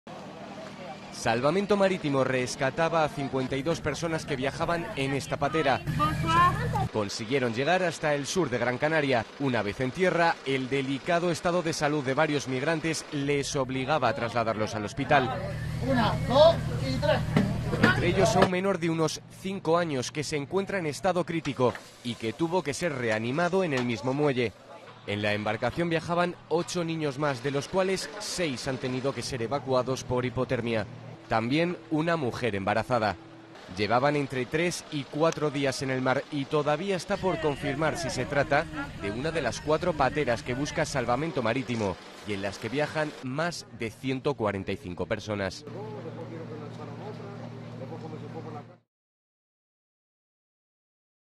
Odisea en una patera, con seis niños ingresados, entre ellos uno con parada cardíaca
Diez de los 52 inmigrantes subsaharianos rescatados la noche del martes a doce kilómetros al sur de Gran Canaria por Salvamento Marítimo fueron derivados a hospitales debido a su mal estado de salud. Seis de ellos son menores de edad y uno ingresó en estado crítico tras sufrir una parada cardíaca, informaron fuentes de los servicios de emergencia.

Fuente: ATLAS